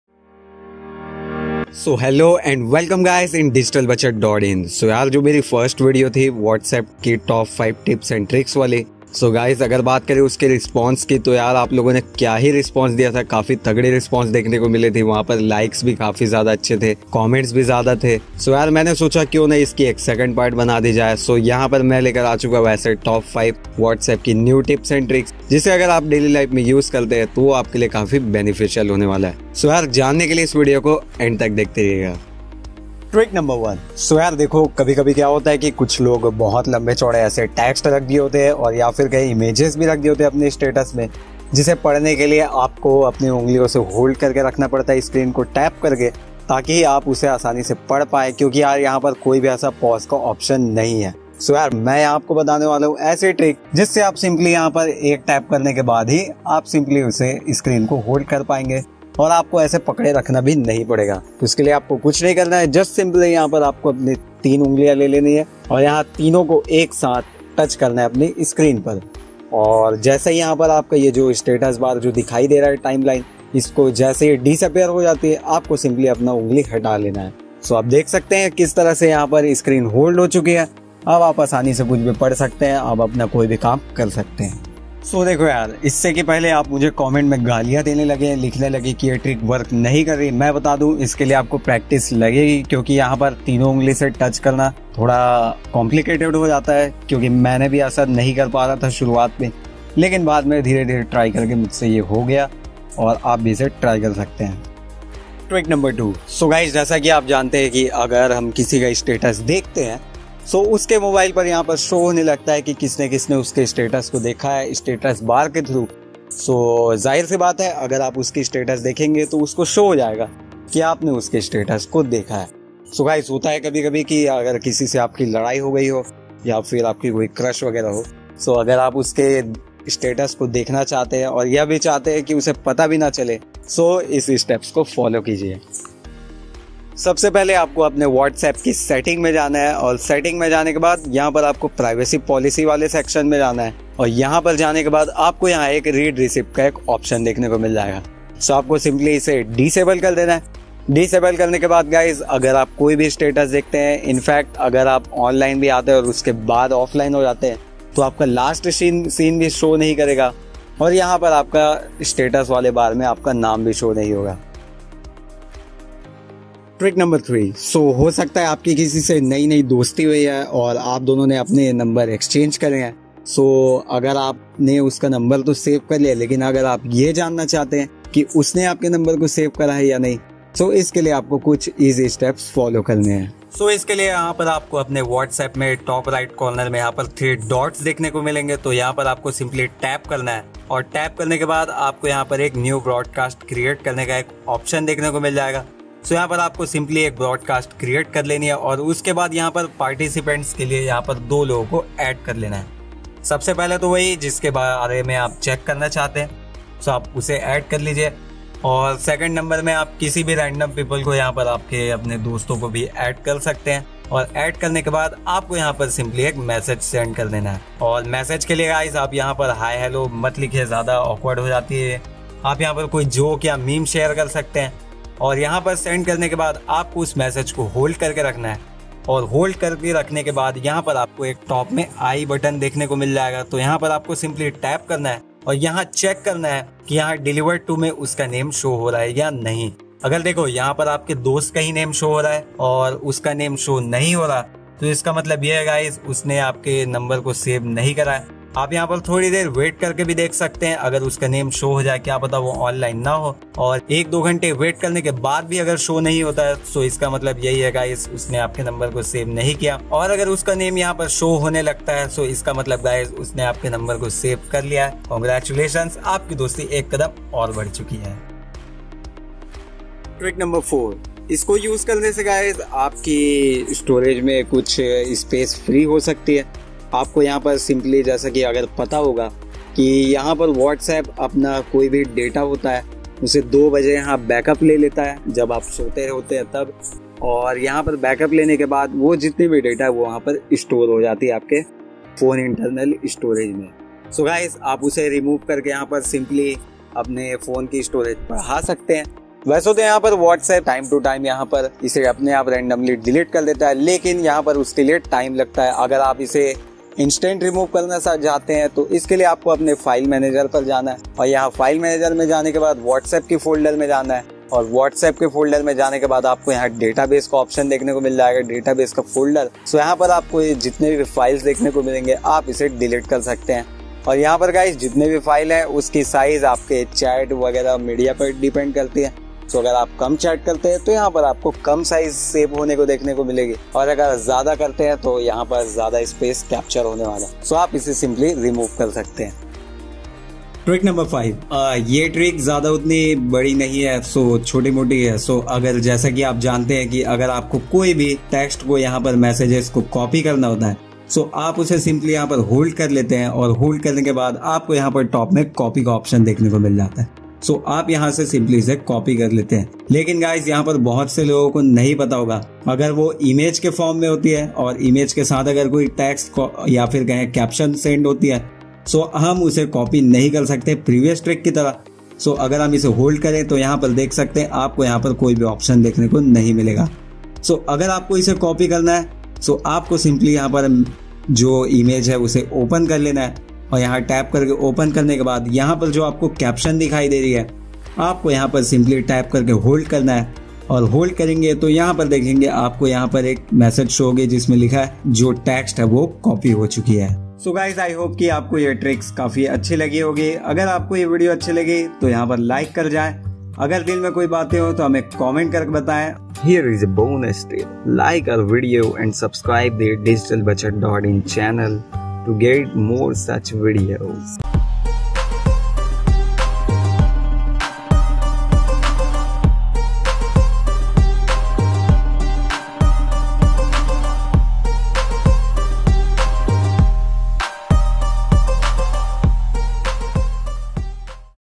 New Whatsapp Secret Tricks and Tips | Whatsapp New Features [Whatsapp Tricks 2020]
New Whatsapp Secret Tricks and Tips | Whatsapp New Features | Whatsapp Tricks 2020 | Digital Bachat

Hey Guys, Is video me bataya gaya hai Whatsapp ke new secret tricks and tips. Agar aap in Whatsapp Tricks and Tips, Features ka use karte hai toh aap ka whatsapp use karne ka experience kaafi achha hone wala hai. Watch this New Whatsapp tricks and features till end.
is video mein meine popular android app whatsapp ke kuch bahot hi important aur kaam aane waale tips & tricks ke baare mein bataya hai jo aapke hamesha kam aayenge.

Digital Bachat's How To Series, where we share tips and tricks about Android, iOS and PC.